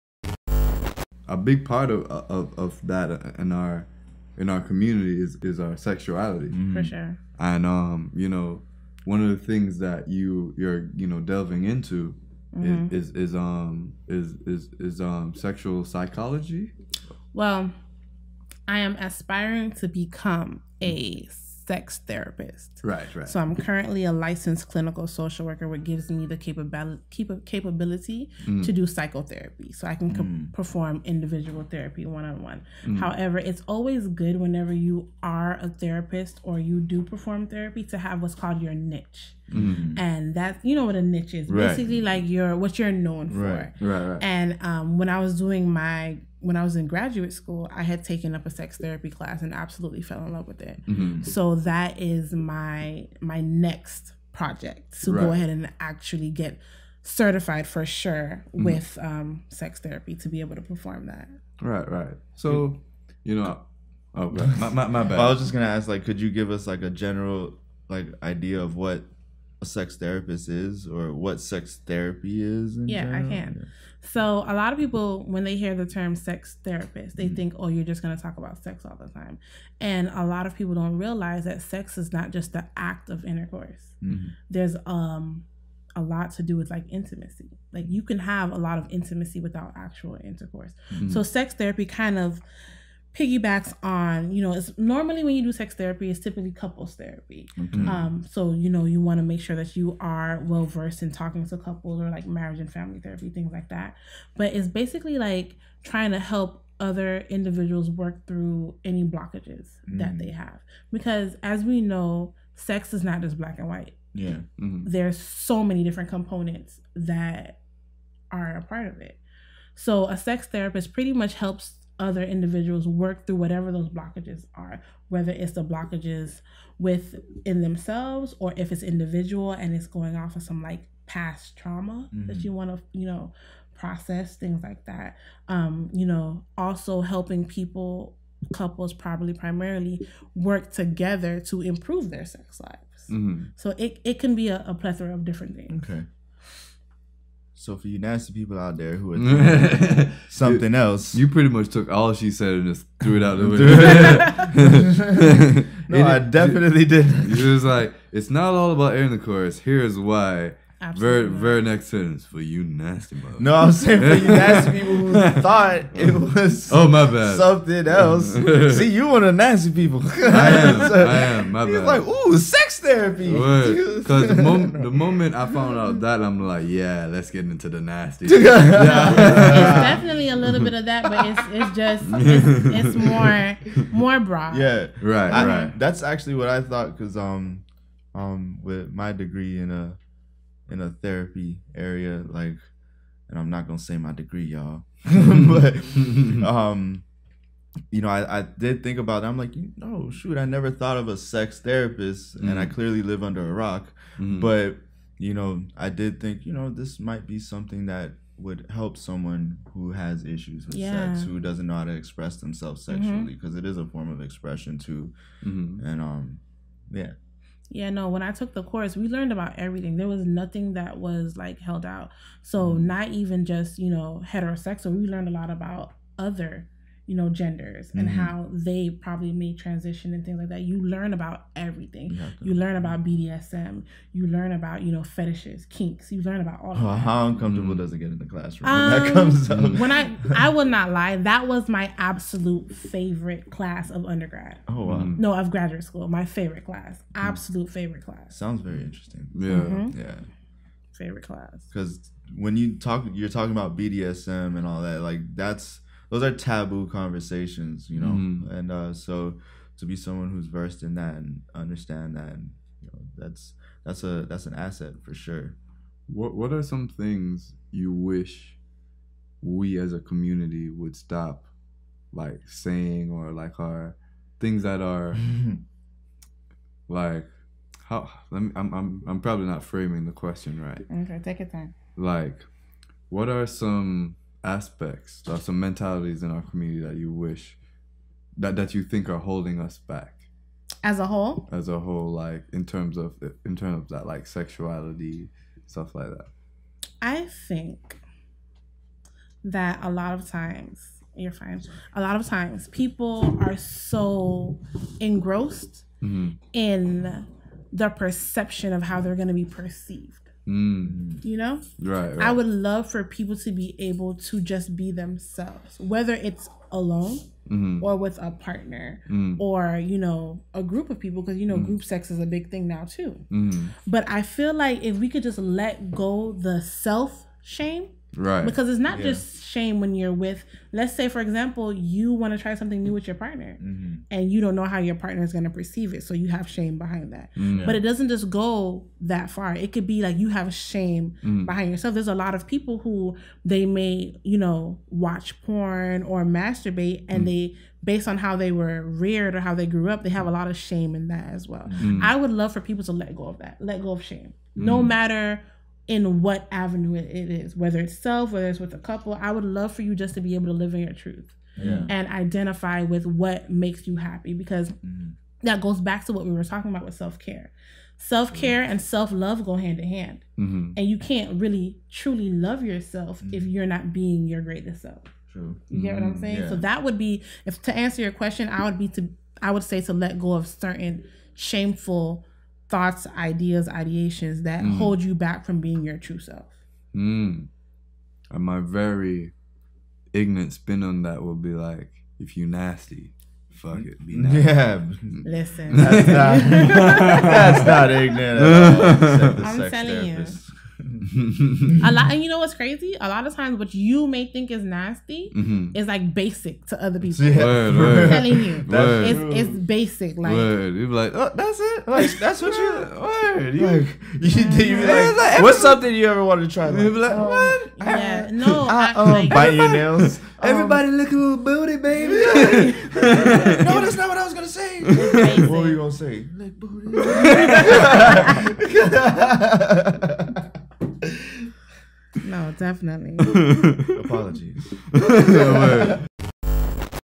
TPSG Ep. 109 - (Shorts) Black Sexuality
In our latest episode, we discuss Minority Mental Health Month, Black Sexuality, Semen Retention (Edging), Mermaid Backlash and we interview Telly Tellz.
Please rate & review us on iTunes!